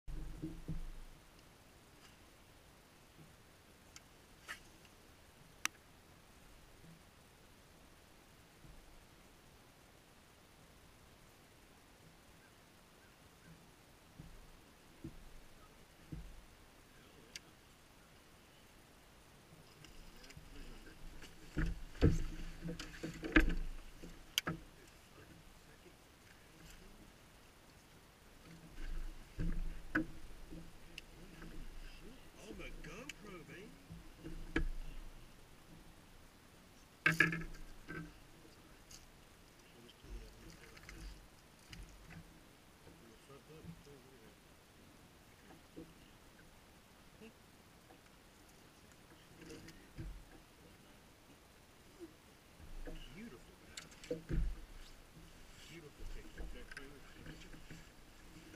Lake Pickwick Bass
Just me catching a fish. That is all.